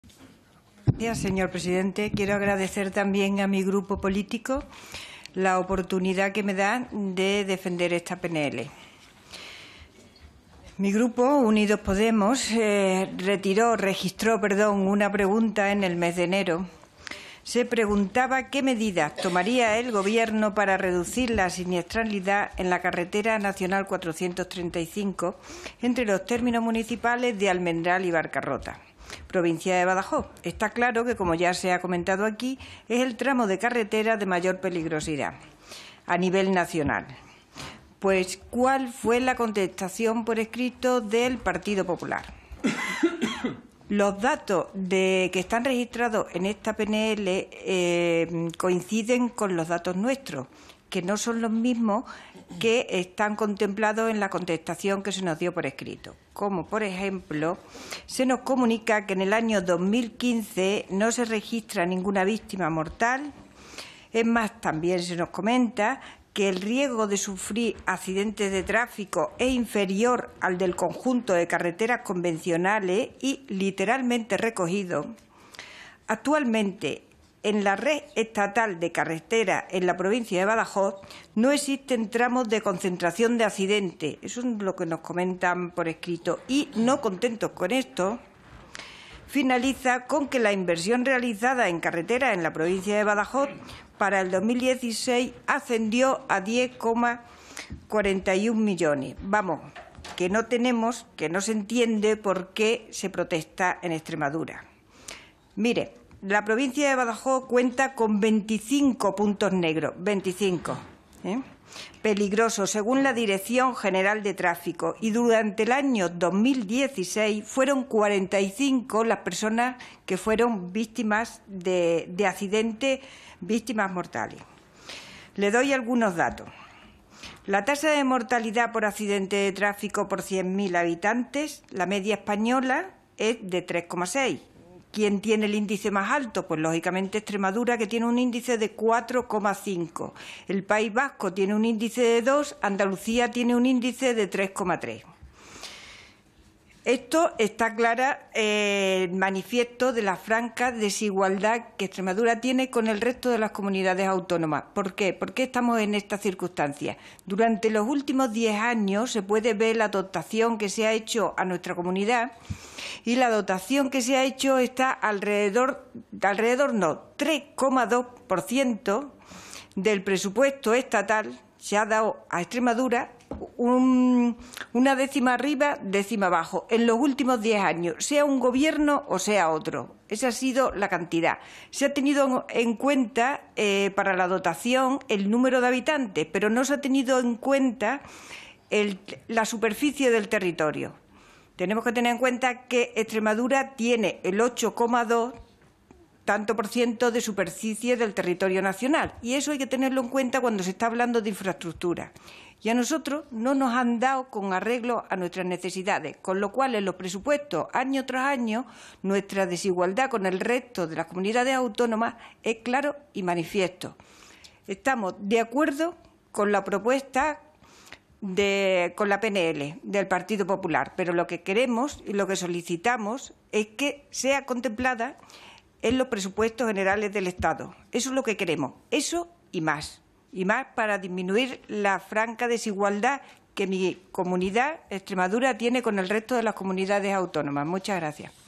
Amparo Botejara en la Comisión sobre Seguridad Vial y Movilidad Sostenible el 21 de septiembre
En el debate de la PNL sobre la mejora de la seguridad de la carretera N-435 en su tramo entre Almendral y el principio de la variante de Barracota.